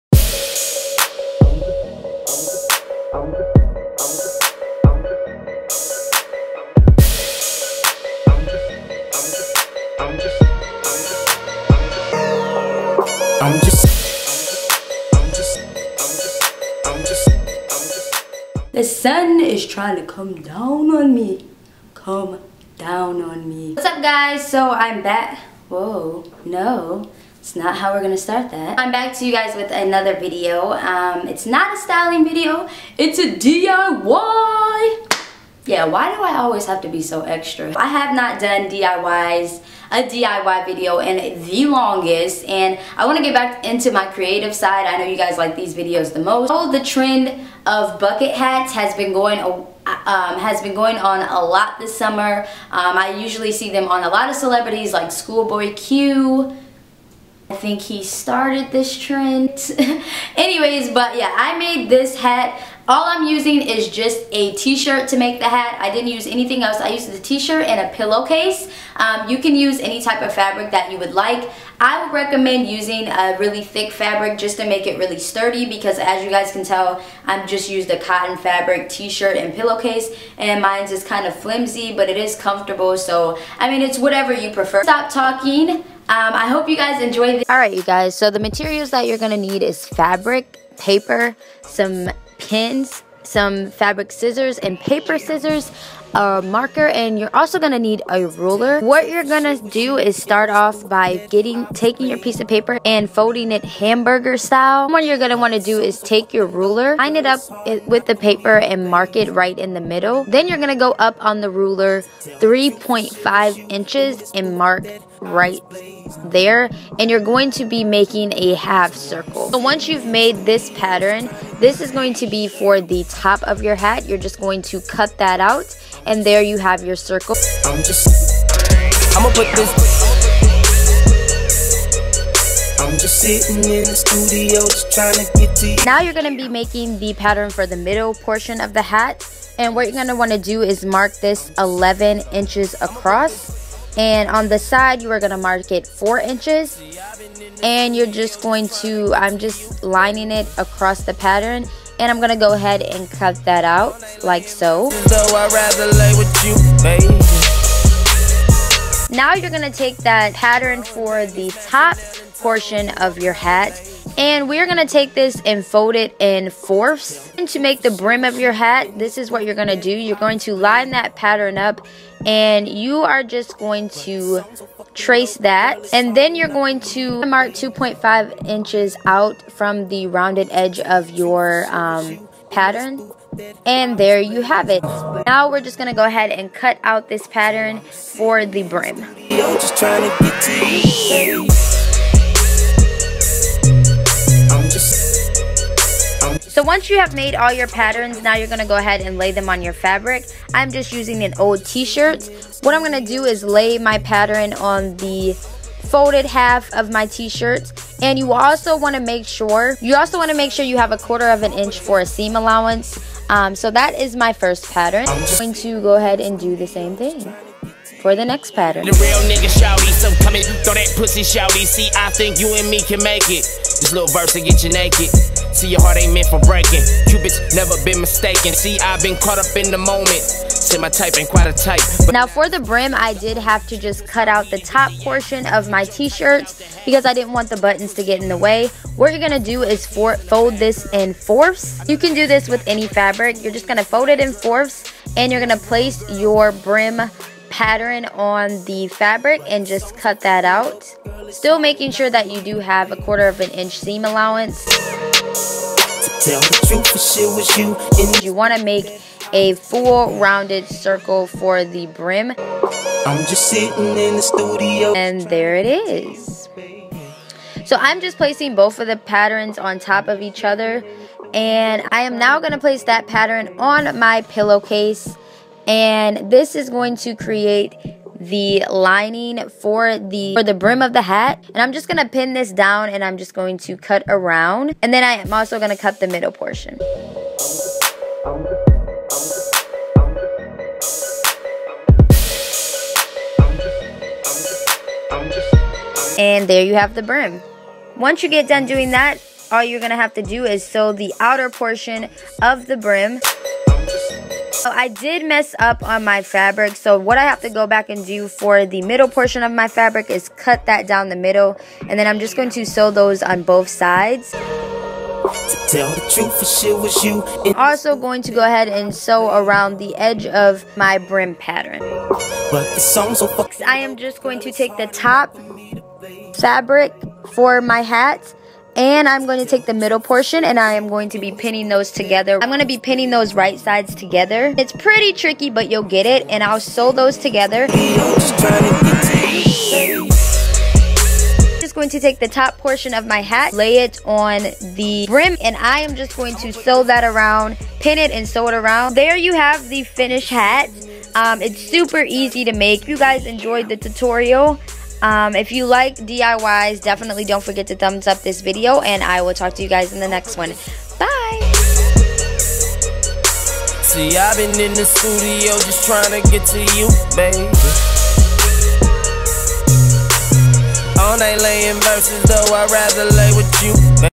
DIY | BUCKET HAT
It's DIY Time!!! So the bucket hats have been trending. Everywhere I go I been seeing people rock these hats. So I decided instead of going out and buying one just to make my own personalized bucket hat. I hope you guys enjoy! If you make a hat TAG me in a photo of you wearing your bucket it on IG. HAPPY SEWING! :) Lubb you!
Takara
xox

Don't forget to "LIKE" for more FASHION DIYS

T-shirt Fabric - Thrift Store
Pillow Case- Walmart

Nerd Stuff! (Filming Equipment)